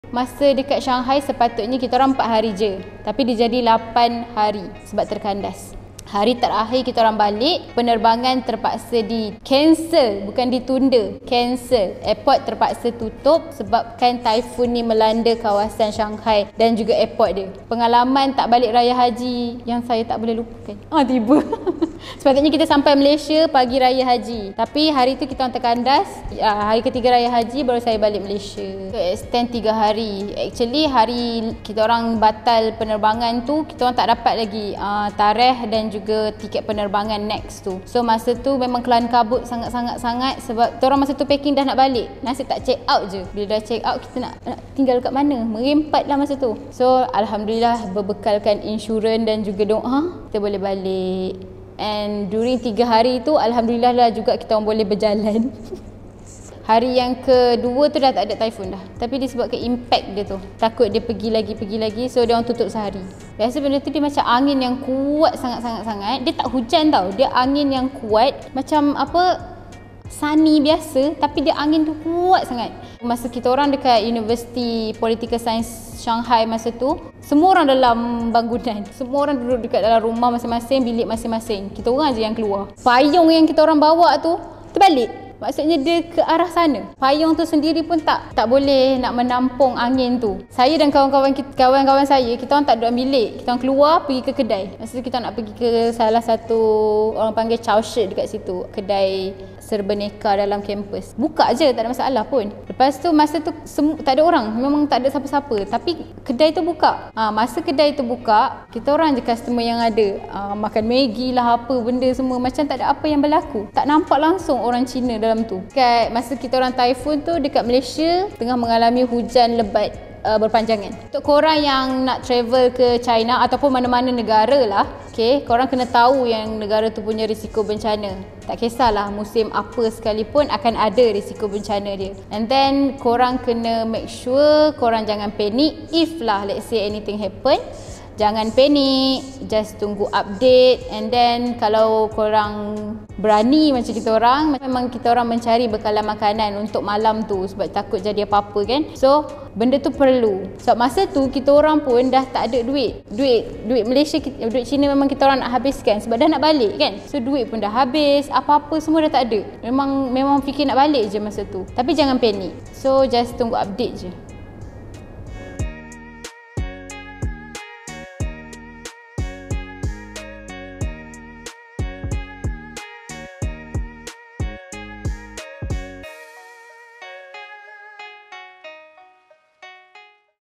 Terkandas di Shanghai Sebab Taufan
Nasib baik belum naik flight. Imagine kalau dah kat atas.